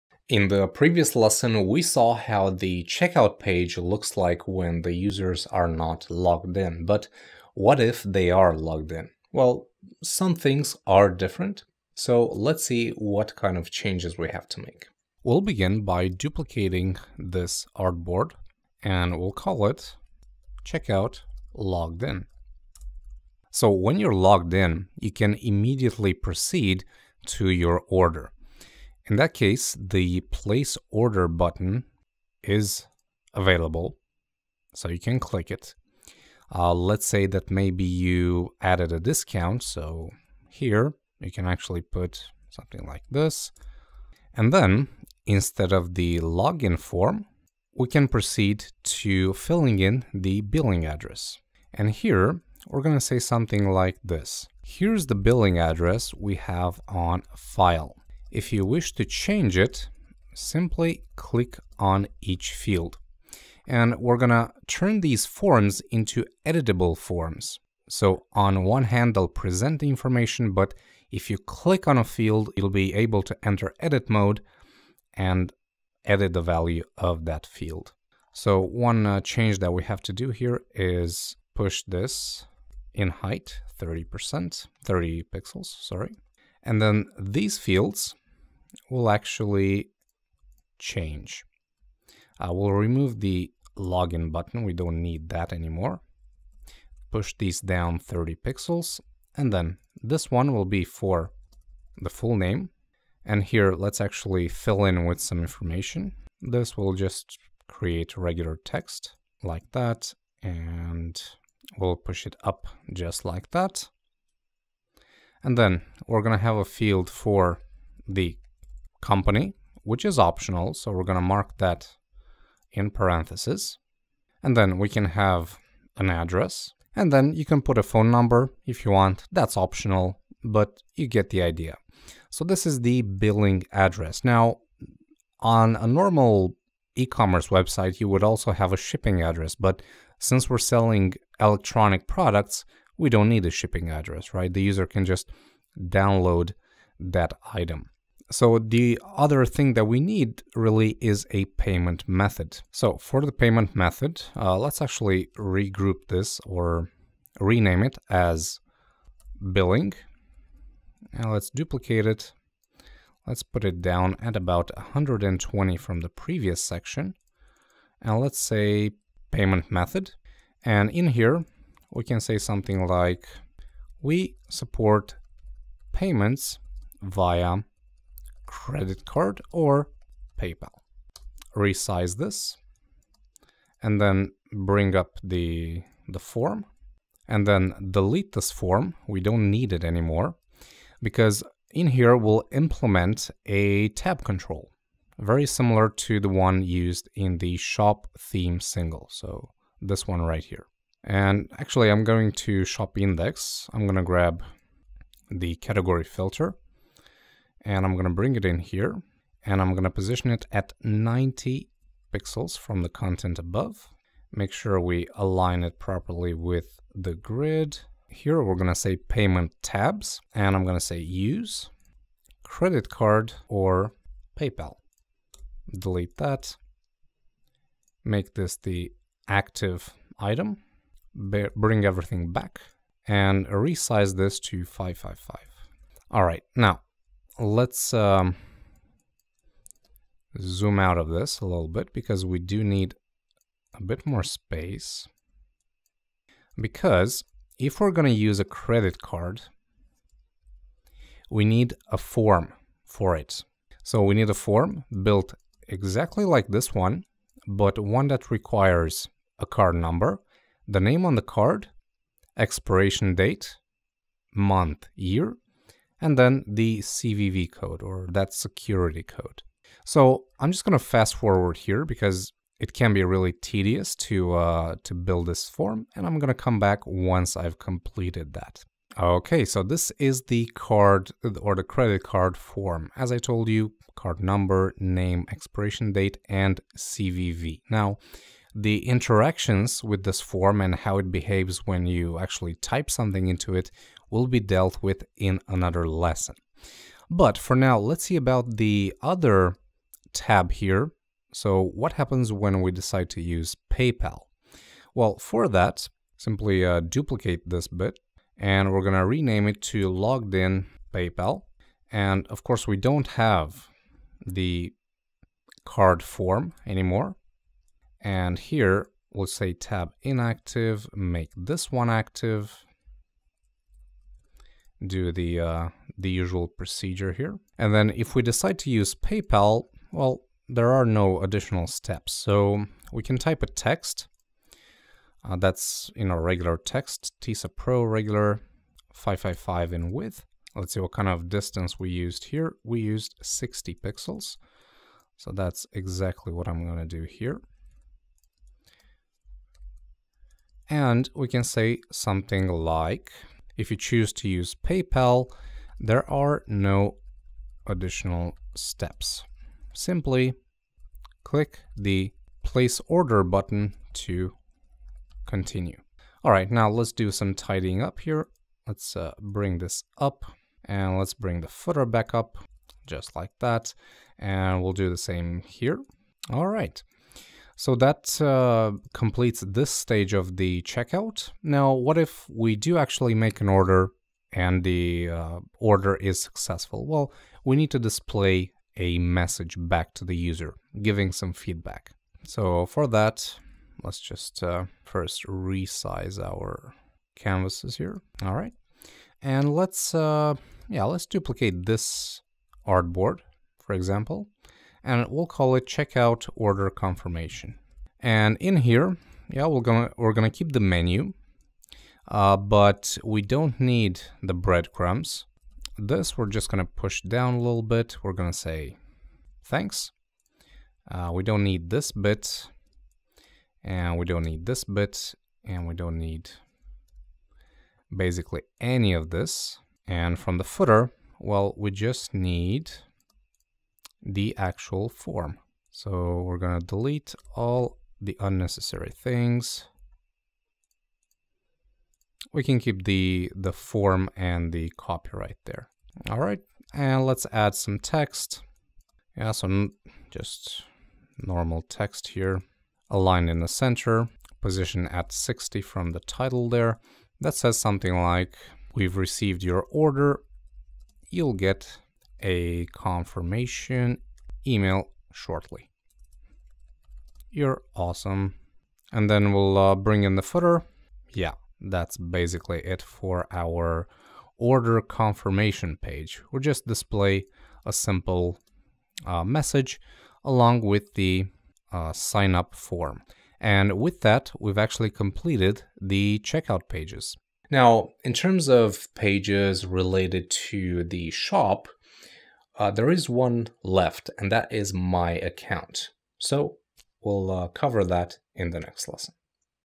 Designing an eCommerce Portfolio: Complete Checkout
In the previous lesson we saw how the checkout pages look like when the users are not logged in. What if they are? Well, there are a few changes so let's see what those are.